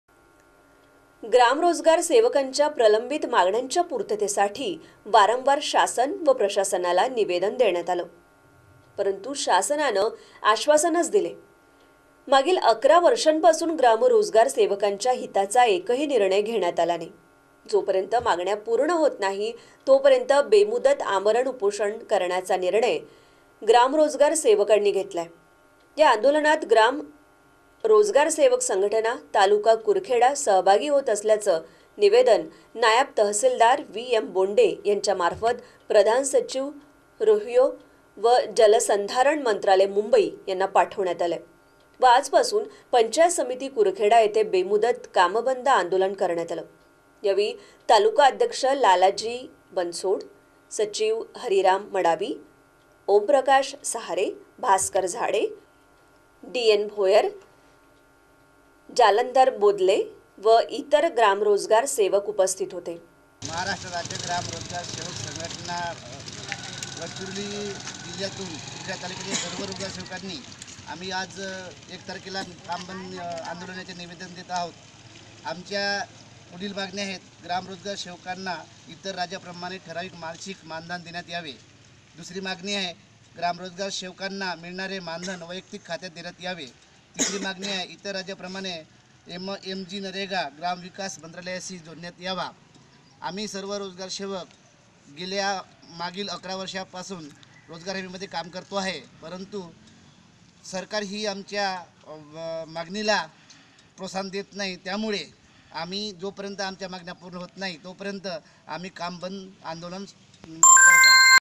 1 January 2019 Search Tv Chandrapur - ग्रामरोजगार सेवकांचे कुरखेडा पंचायत समिती
ग्रामरोजगार सेवकांचे कुरखेडा पंचायत समिती समोर बेमुदत कामबंद आंदोलन सुरु